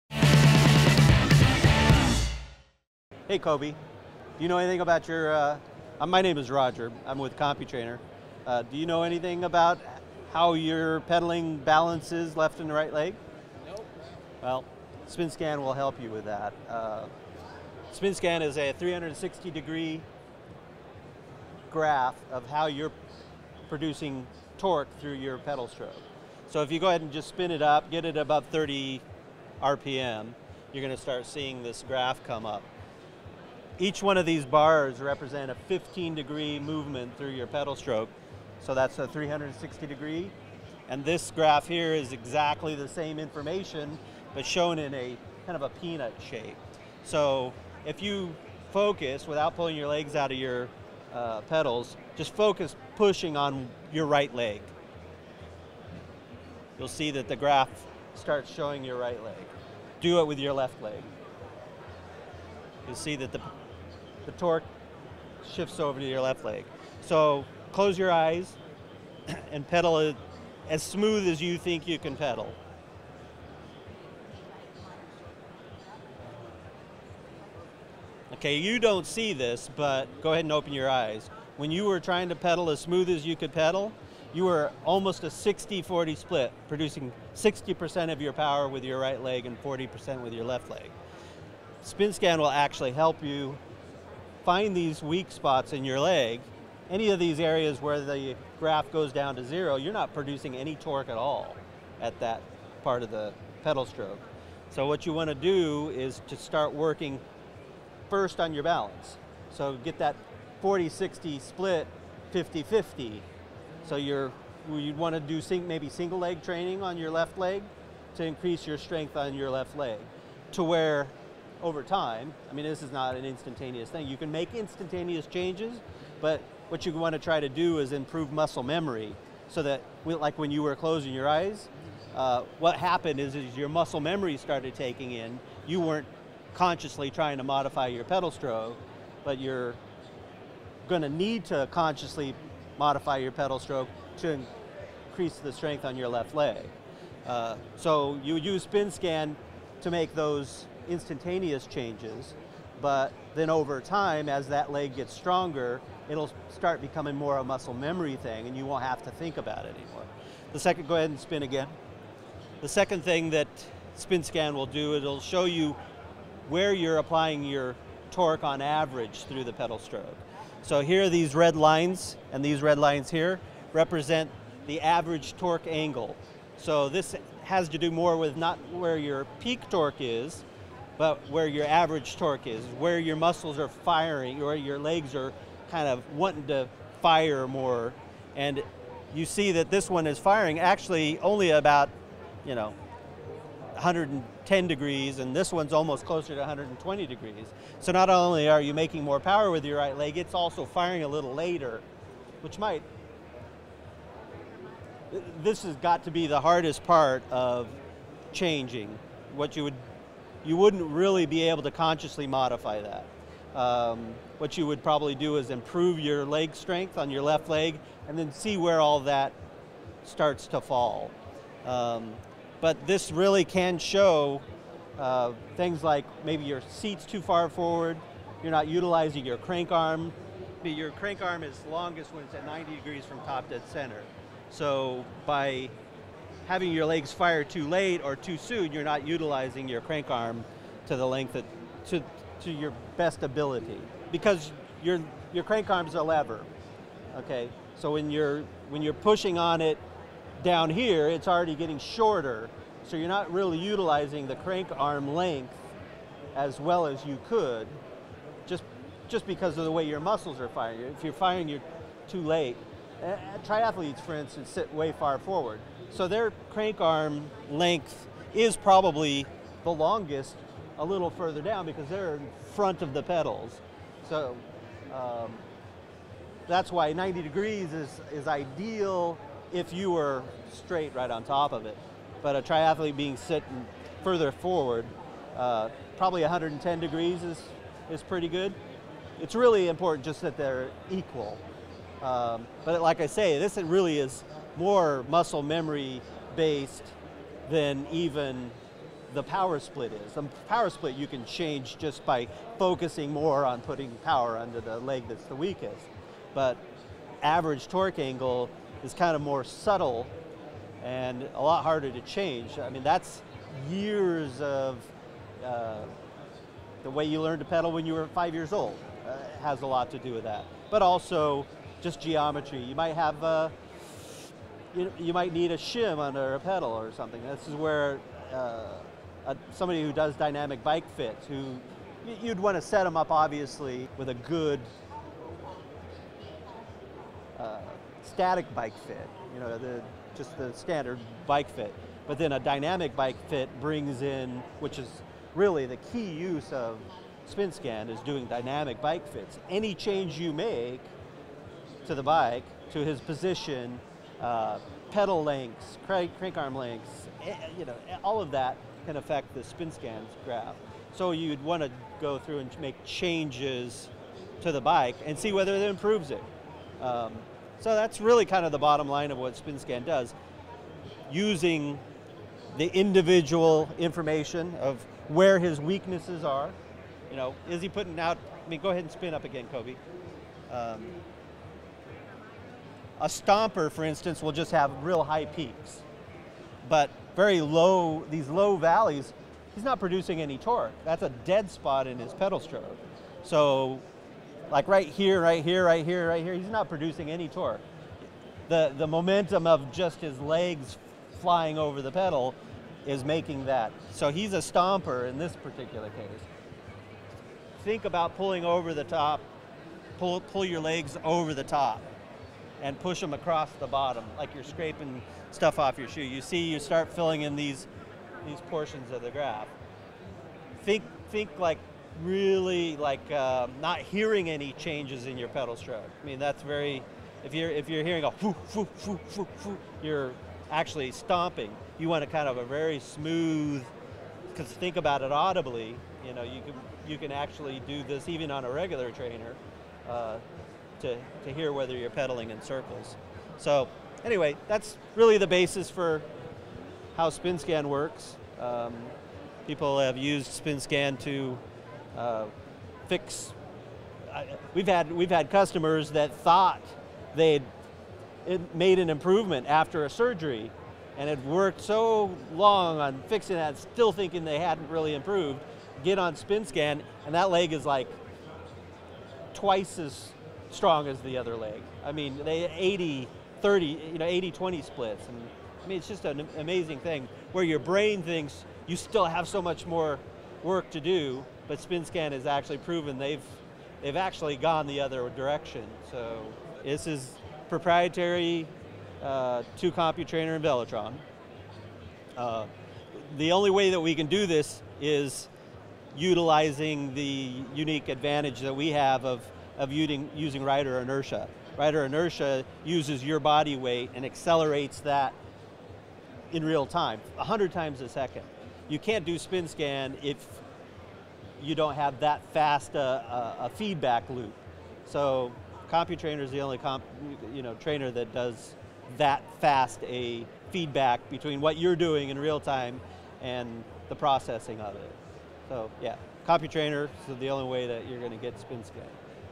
How a Computrainer Can Improve Your Cycling
Earlier this year I met with Roger from RacerMate, who shared with me how Computrainer can great improve my efficiency on the bike.